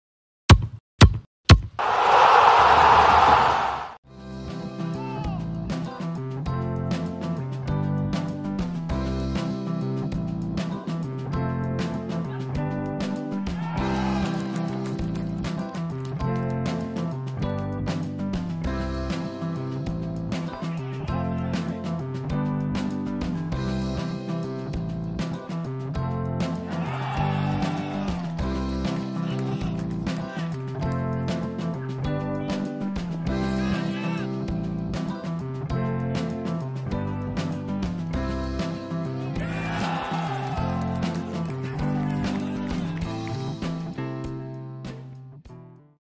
SK Rudolfov – SK Mirovice 3:0
Sestřih gólů z utkání 1. kola KP Rudolfov vs. Mirovice.